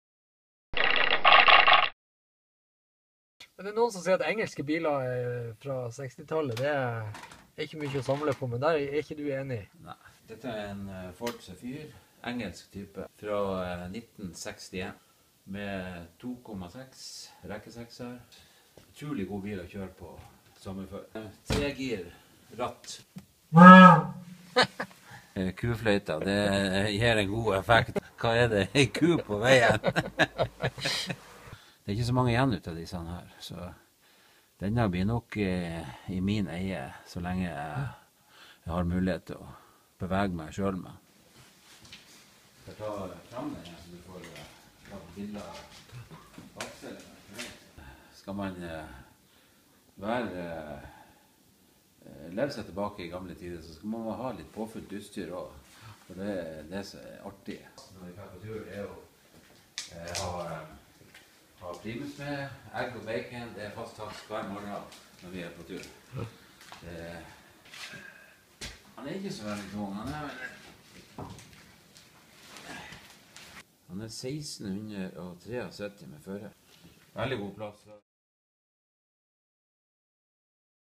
Garasjebesøk på Sortland
Aarstein på VOL på besøk hos Tore Geir Olsen i Vesterålen motorhistoriske forening. Det er klart for vårpuss...